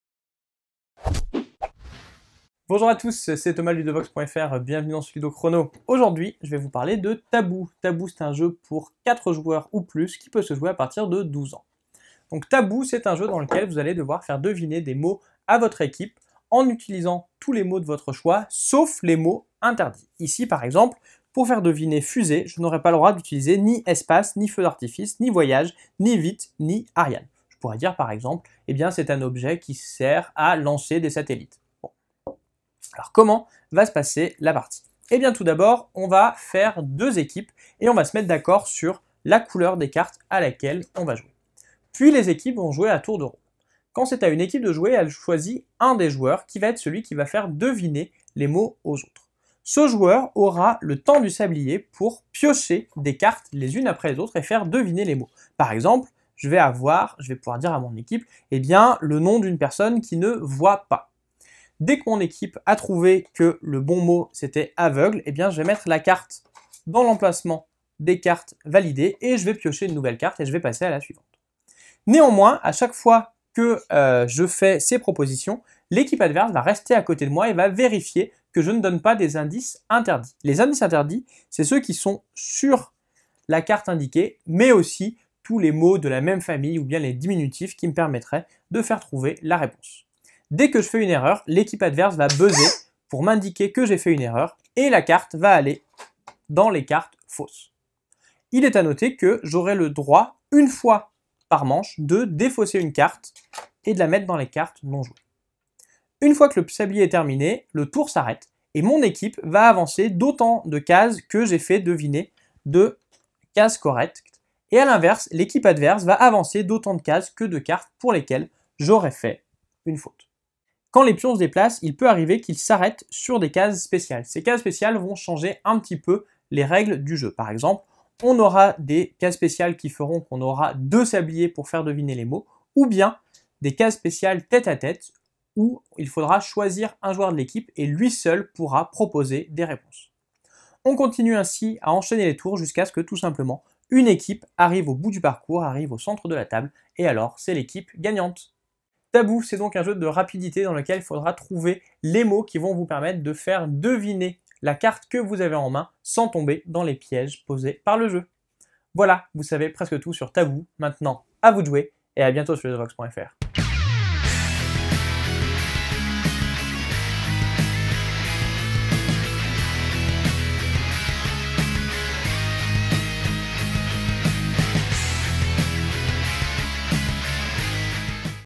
Ludochrono - Taboo
Voici la présentation du jeu "Taboo" avec une vue d'ensemble du fonctionnement et du matériel, le tout en 5 minutes.